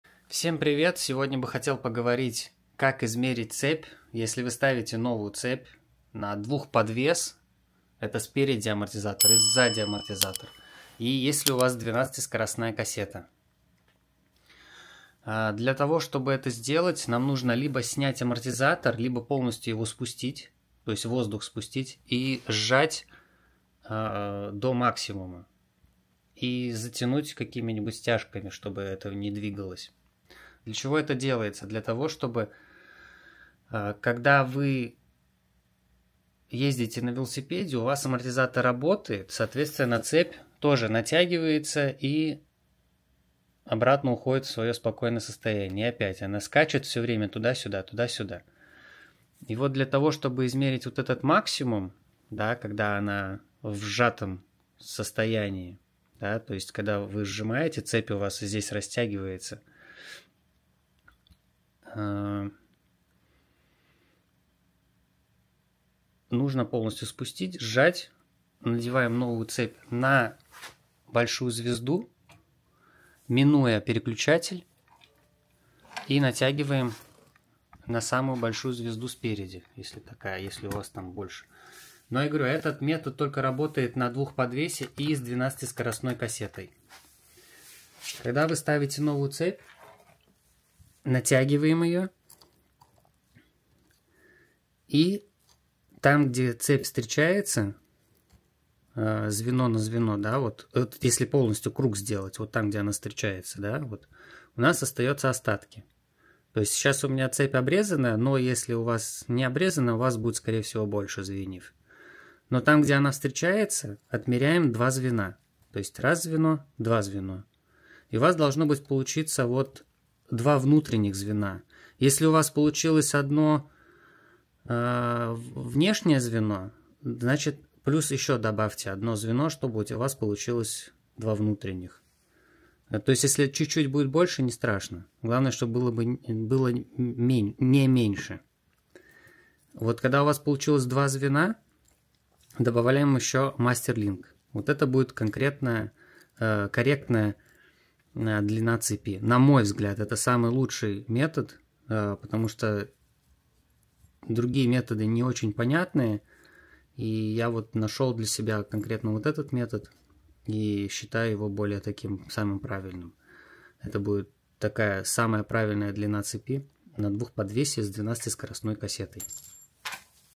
Как правильно измерить цепь на двухподвесе с 12 скоростной кассете?
Как подобрать длину цепи велосипеда?